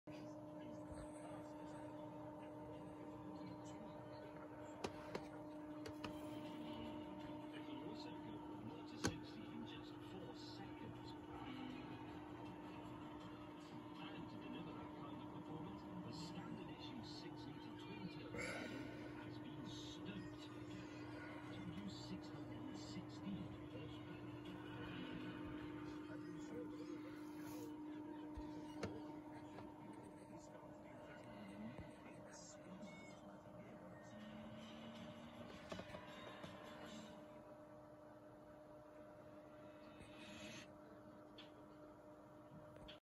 Asus ROG Strix GA15DH with upgraded fans inc front fan and RGB ram
So far I have changed the cpu cooler for an AMD Wraith Prism cooler with RGB , I have also changed the stock rear 92mm exhaust fan for Noctua NF-A9 FLX 92mm premium fan and as well as this I have also added a Noctua NF-A12x25 PWM 120mm premium fan to the front of the case as an intake. The airflow through this case is now hugely improved.  I’ve also now added G.Skill Trident Z Neo 32GB (2x16GB) DDR4 3200MHz Dual Channel Memory (RAM) Kit which also has RGB.